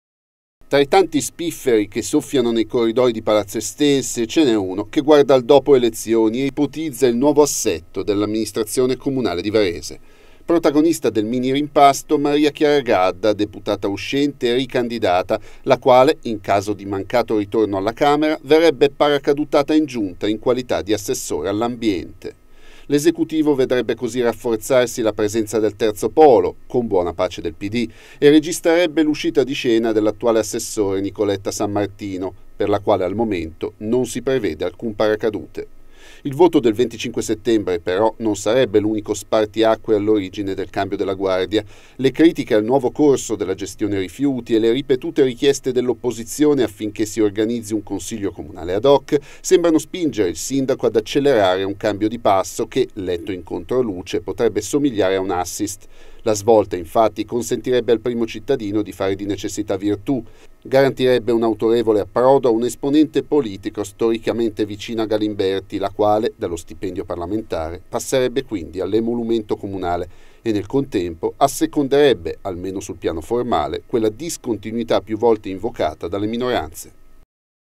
Varese nel dopo Elezioni (con Gadda in giunta)
Da tempo le indiscrezioni più diffuse a Palazzo Estense prevedono che, in caso di mancata elezione in Parlamento, l'esponente del Terzo Polo entri nell'esecutivo in qualità di Assessore all'Ambiente, al posto di Nicoletta San Martino .